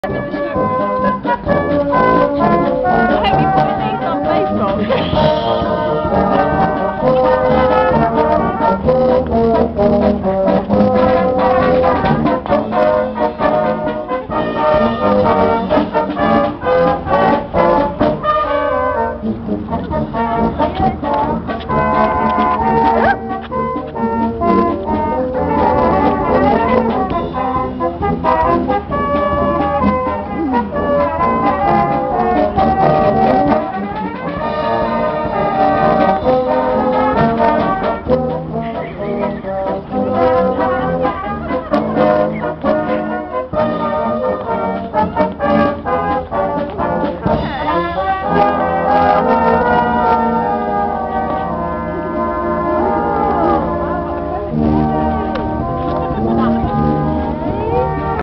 Vernon Bear 1
Ben Stroud as Vernon Bear (Stockport County's old mascot) with the Vernon Brass Band at the Vernon Building Society's open day